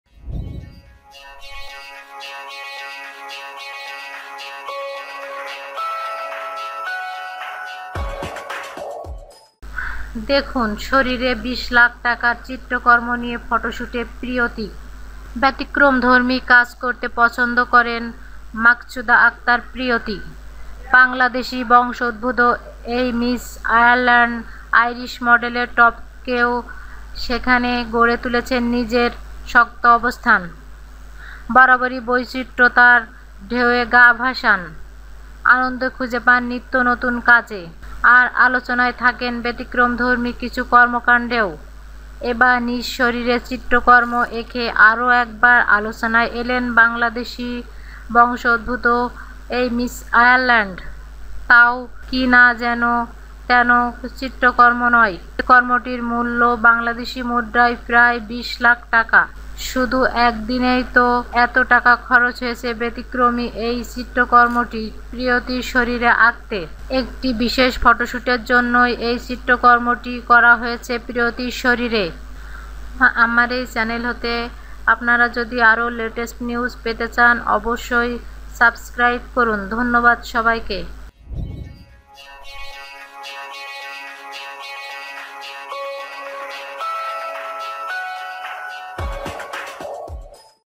দেখুন শরীরে ২০ লাখ টাকার চিত্রকর্ম নিয়ে ফটোশুটে প্রিয়তি
ব্যাতিক্রমধর্মী কাজ করতে পছন্দ করেন মাকসুদা আক্তার প্রিয়তি। বাংলাদেশি বংশোদ্ভূত এই মিস আয়ারল্যান্ড আইরিশ মডেলদের টপকেও সেখানে গড়ে তুলেছেন নিজের শক্ত অবস্থান।

বরাবরই বৈচিত্র্যতার ঢেউয়ে গা ভাসান। আনন্দ খুঁজে পান নিত্য নতুন কাজে। আর আলোচনায় থাকেন ব্যতিক্রমী কিছু কর্মকাণ্ডে।
এবার নিজ শরীরে চিত্রকর্ম এঁকে আরও একবার আলোচনায় এলেন বাংলাদেশি বংশোদ্ভূত এই মিস আয়ারল্যান্ড। তাও কী না যেন তেন শিল্পকর্ম নয়। যে কর্মটির মূল্য বাংলাদেশি মুদ্রায় প্রায় ২০ লাখ টাকা। শুধু একদিনেই এতো টাকা খরচ হয়েছে ব্যতিক্রমী এই চিত্রকর্মটি প্রিয়তির শরীরে আঁকতে।
একটি বিশেষ ফটোশুটের জন্যই এই চিত্রকর্মটি করা হয়েছে প্রিয়তির শরীরে। বিশেষ এই ফটোশুটটি করেছেন আইরিশ সেলিব্রিটি স্থপতি আলফ্রেড কোচরেন ও তার টিম। আর ছবিটি তুলেছেন নোলেন এবং এডি। বডি পেইন্টিংয়ের আর্টিস্ট ছিলেন জার্মানির বিয়াঙ্কা। এছাড়া ফুলের কারুকাজ সজ্জিত এই পোশাকটি বিশেষভাবে আনা হয়েছে হলিউড থেকে।